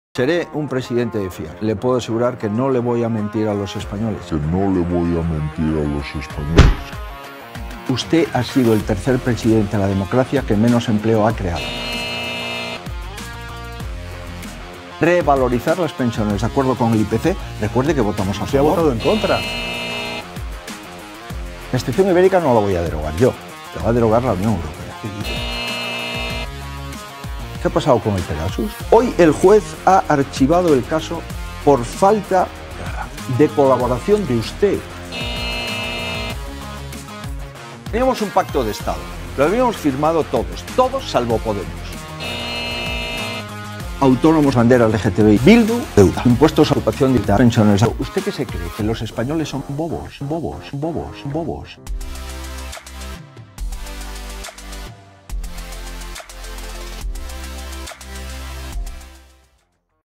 ¿Cuál es la credibilidad del señor Feijóo?
Os resumimos las mentiras (sí, fueron muchas) que soltó en el CaraACaraAtresmedia